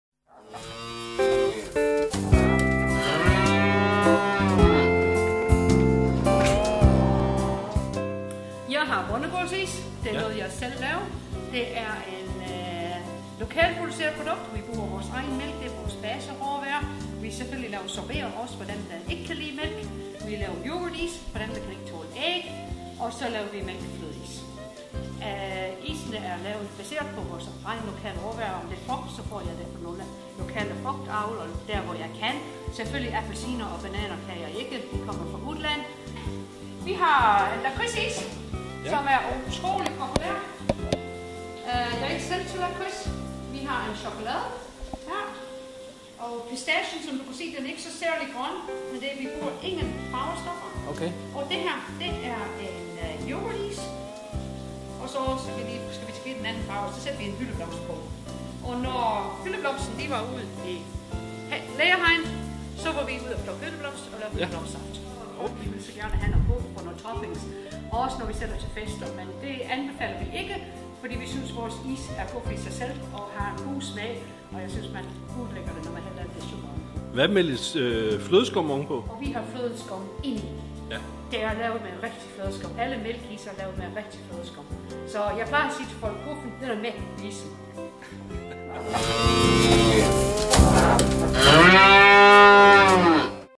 Hveddegaard is
På Hveddegaard i Kibæk laves is på mælk fra egne køer og råvarer fra lokalområdet.